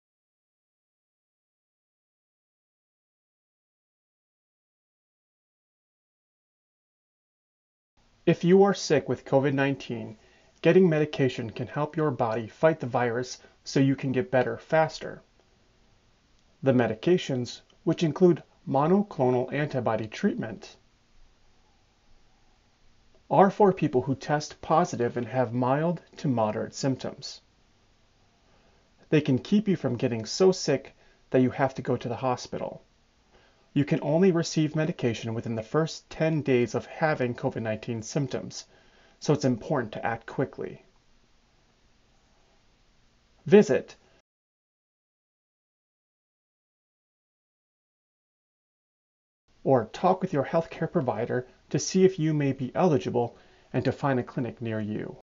Act Quickly To Receive COVID-19 Medications
Transcript:
If you are sick with COVID-19, getting medication can help your body fight the virus so you get better faster.
The medications, which include monoclonal antibody treatment, are for people who test positive and have mild to moderate symptoms. They can keep you from getting so sick that you have to go to the hospital.
You can only receive medication within the first 10 days of having COVID-19 symptoms, so it’s important to act quickly.
Visit health.mn.gov/covidmeds or talk with your health care provider